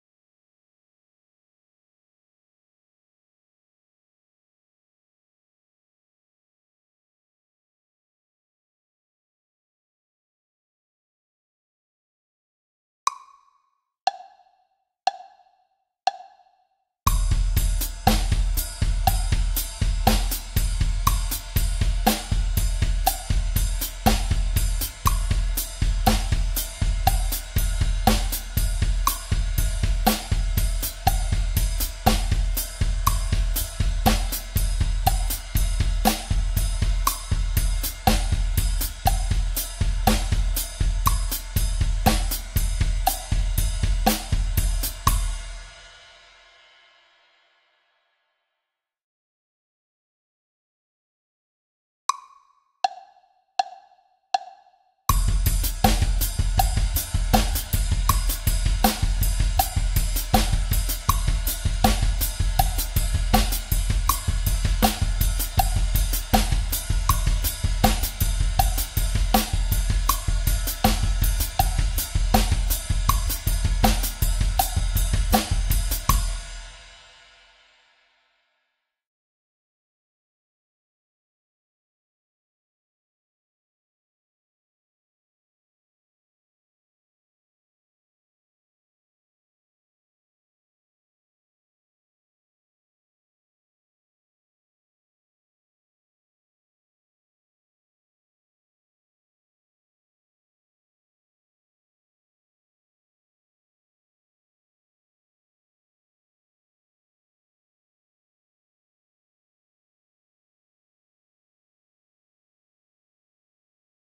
Drum Exercise | Foot Workouts (Part 208 - RRRLRRL) | Panos Geo
Here is Part 208 of the ‘Foot Workouts’ series!

In this video, I keep a steady back-beat with my hands, and play the one hundred and second 7-note pattern (RRRLRRL - right / right / right / left / right / right / left) with my feet, at 60bpm at first, and then a little bit faster, at 80bpm.

The entire series was recorded and filmed at my home studio in Thessaloniki, Greece.

Recording, Mixing, Filming, and Video Editing by Panos Geo

‘Panos Geo’ logo by Vasilis Georgiou at Halo Creative Design Lab